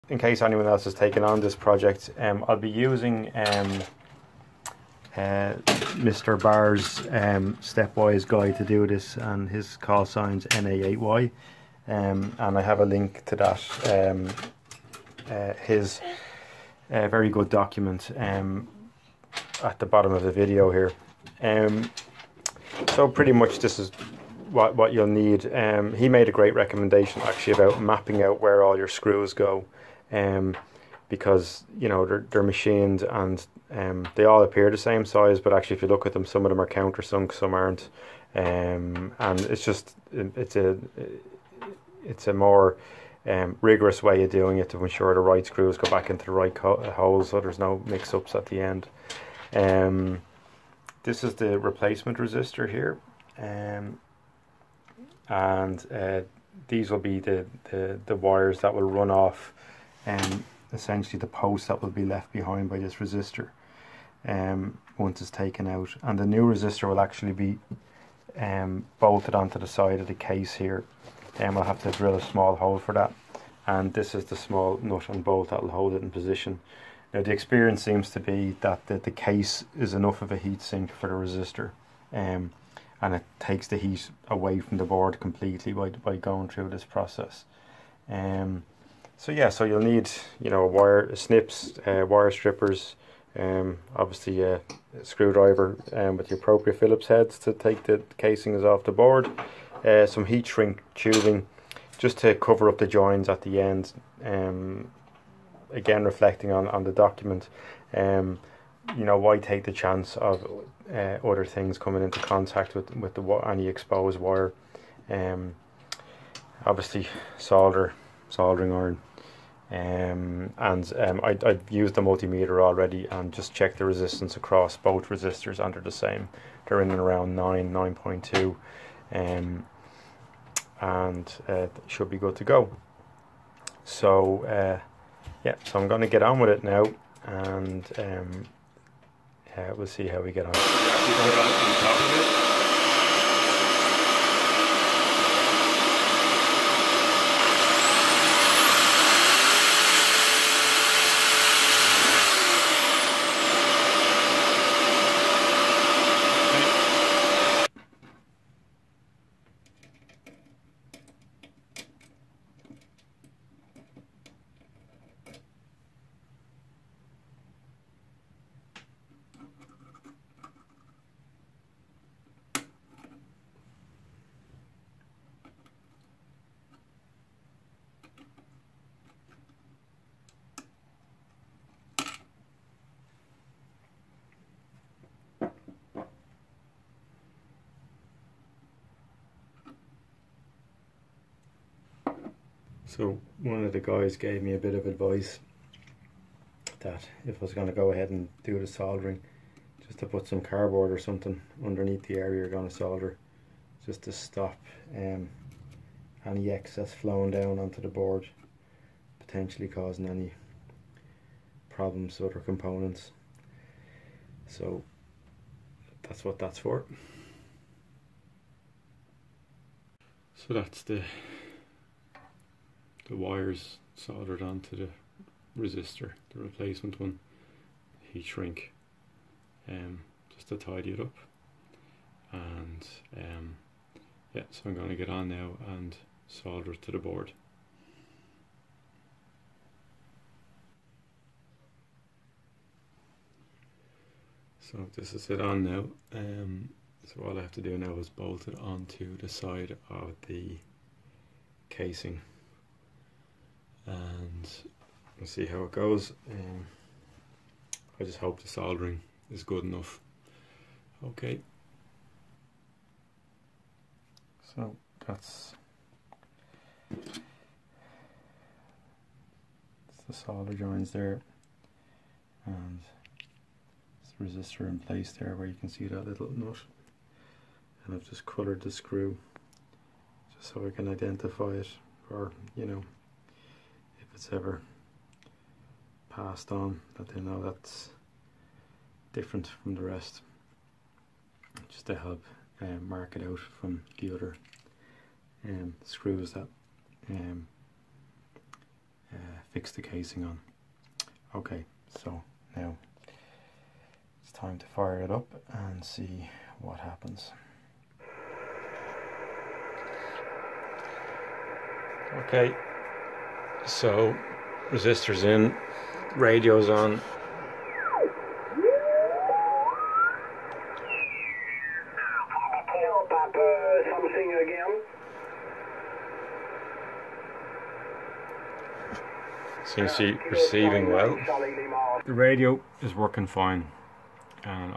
Yaesu FT-840 resistor (R2213) replacement
I was having RFI problems on transmit i.e. S-meter was going off the scale after PTT. I had the rig checked by fellow South Dublin Radio club member. The radio checked out fine in all respects. So I went ahead and swapped out the potentially troublesome R2213 - as I thought this may be the cause of my interference problem.

Post Resistor replacement the radio was operating normally. However, when transmitting on anything greater than 5W, after PTT was released, the S-meter needle still stayed hard right for a period of time.

Answer:
After this video was made, another operator suggested that it could be a PIR e.g. a sensor light, being triggered by RF during transmit.

That's exactly what it was! My own back garden sensor light. I dialled back the sensitivity of the PIR and there now no RF triggering during daylight hours. However, it is still an issue after dark. My plan is to install an internal mains switch for the sensor lights so I can knock them off when using the radio.